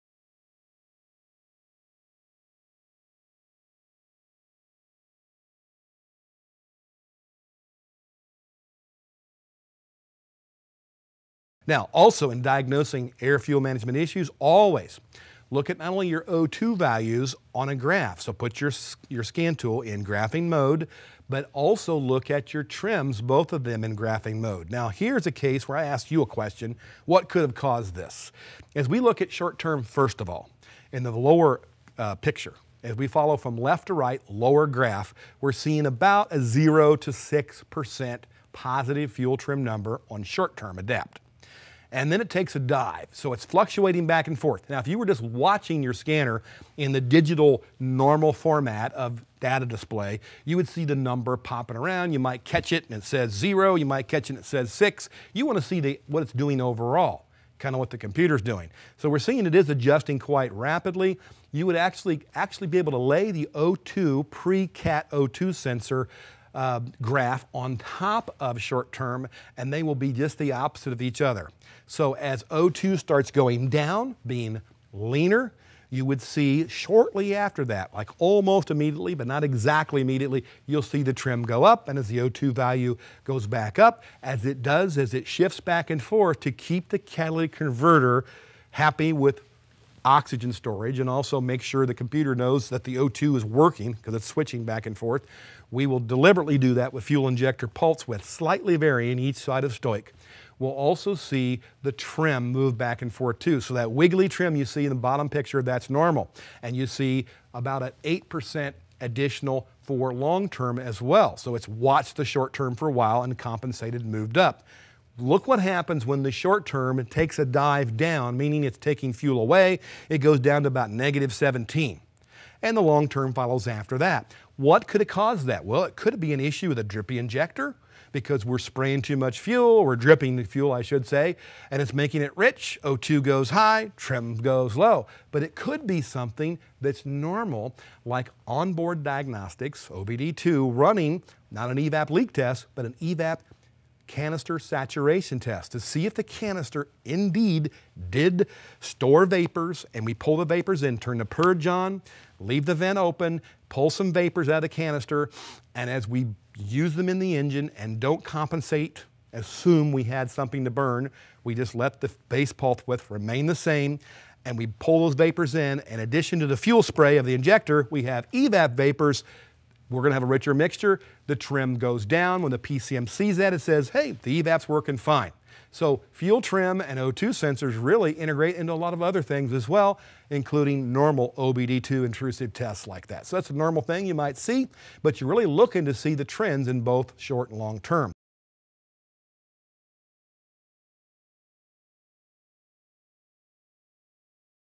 How to Diagnose Air Fuel Management Issues (Diagnosing to Win with Fuel Trim)
What are the steps to take in diagnosing air-fuel management issues? The Delphi Training Series breaks it down for you.